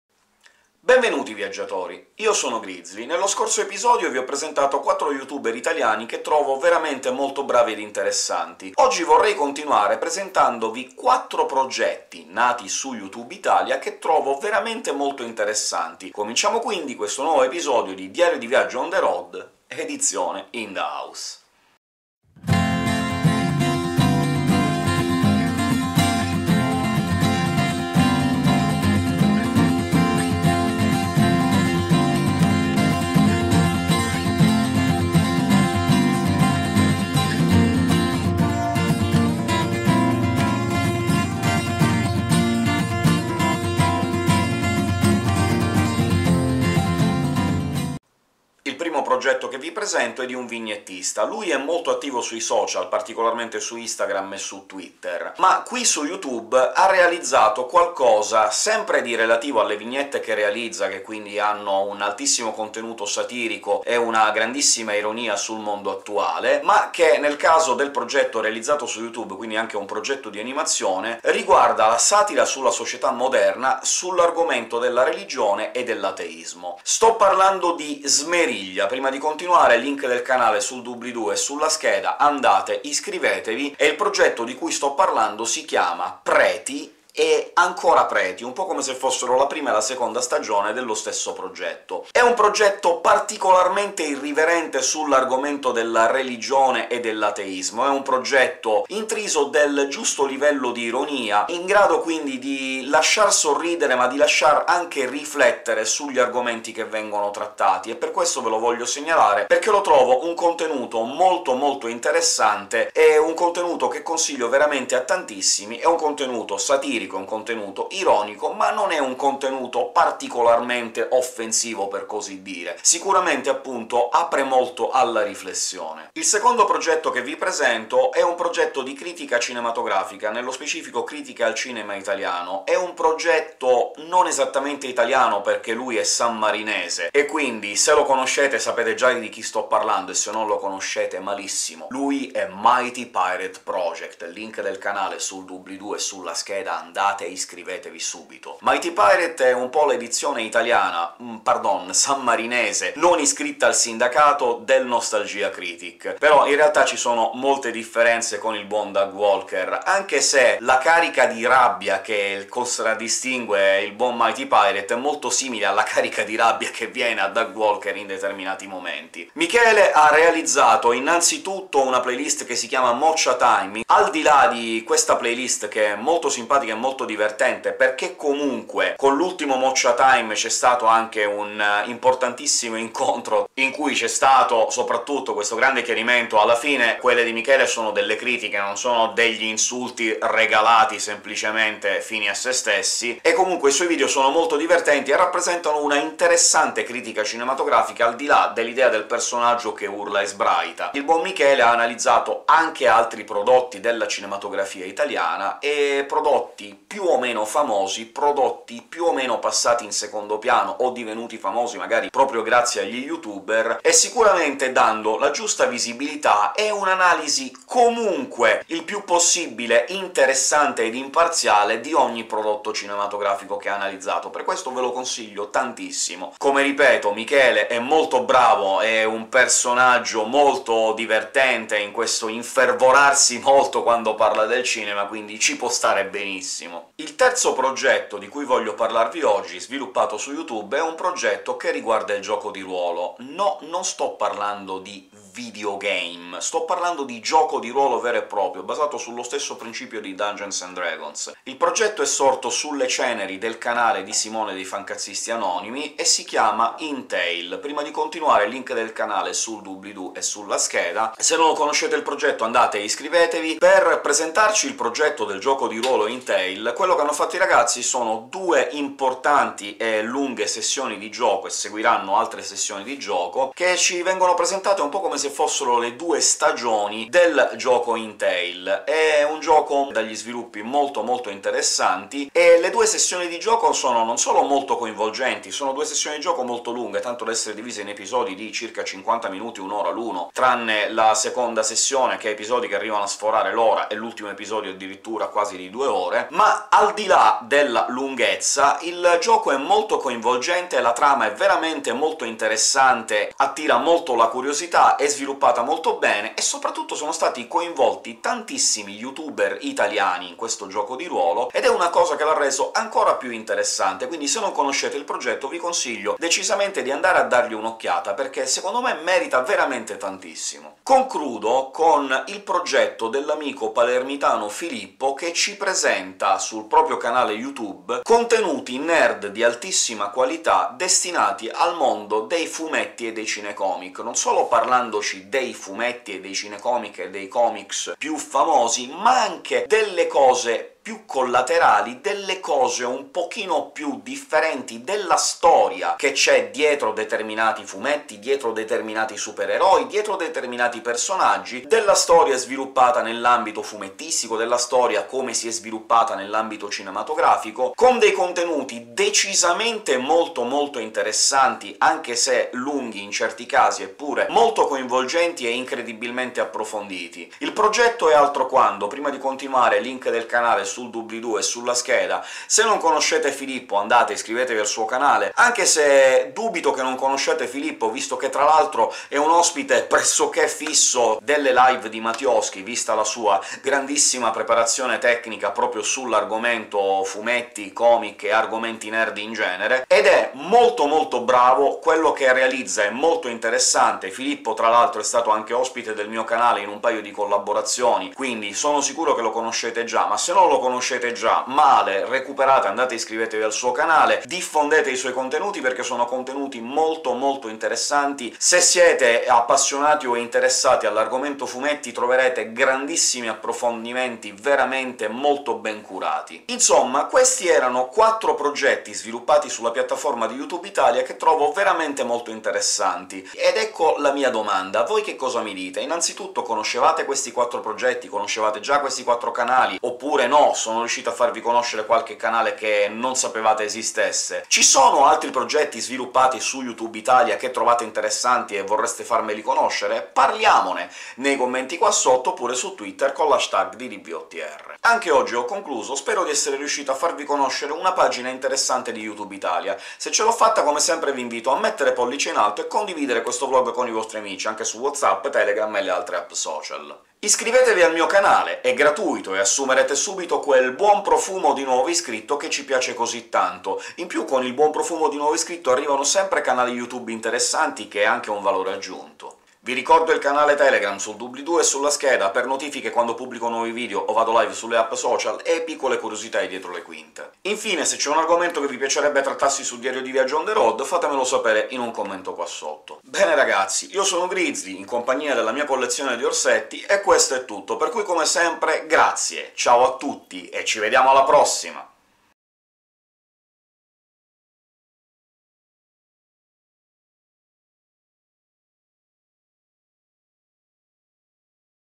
Quattro eccellenti progetti YT italiani - DdVotr 318
Qualcosa di molto carino, ma interessante 😉

= Indice:
0:00 introduzione
0:48 primo progetto
2:06 secondo progetto
3:57 terzo progetto
5:21 quarto progetto
6:56 domanda
7:25 saluti finali